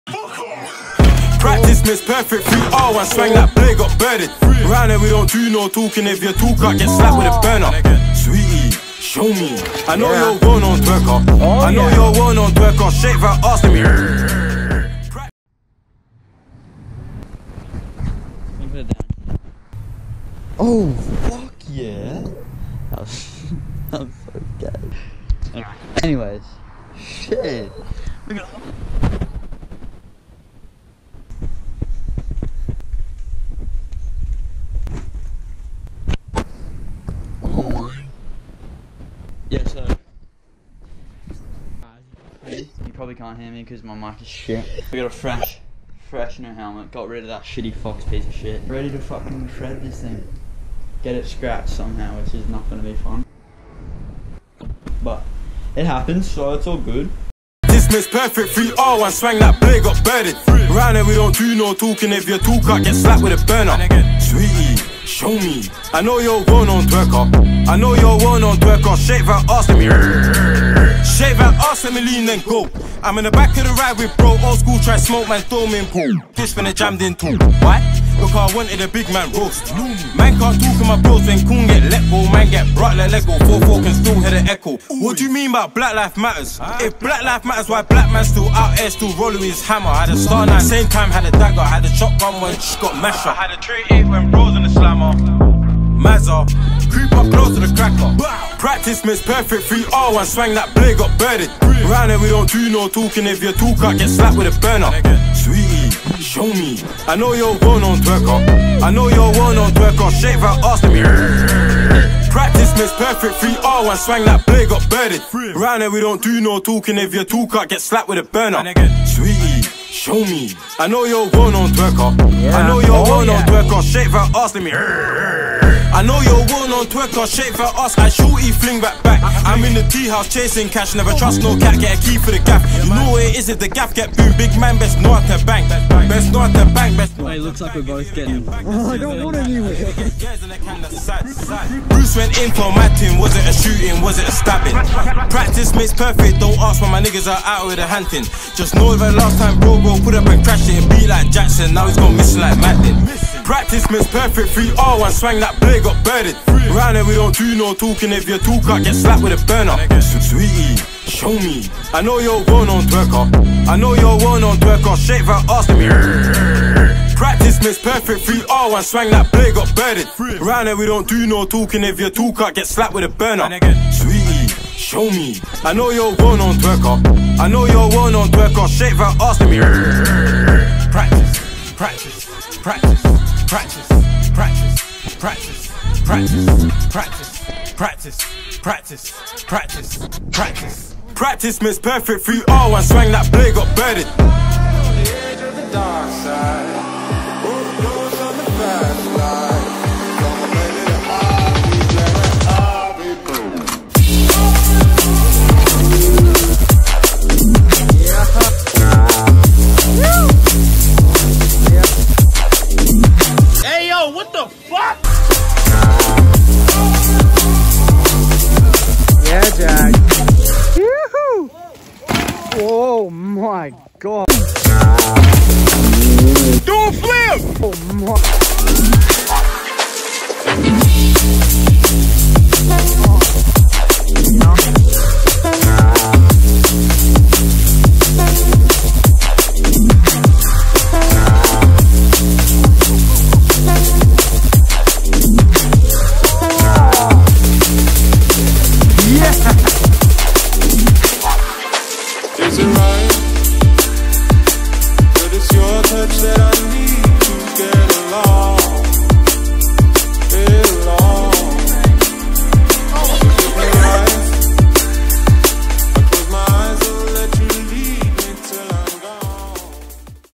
MASSIVE SAVES ON DH RACE LINE (Boomerang Farm)
Got a cheeky new helmet, FOR only $150 absolute steal, it might be second hand but still, how good. Other than that some brissy boys came down for a ride with Oscar and I and we all just had a great time, let me know what you think of my helmet and have a good one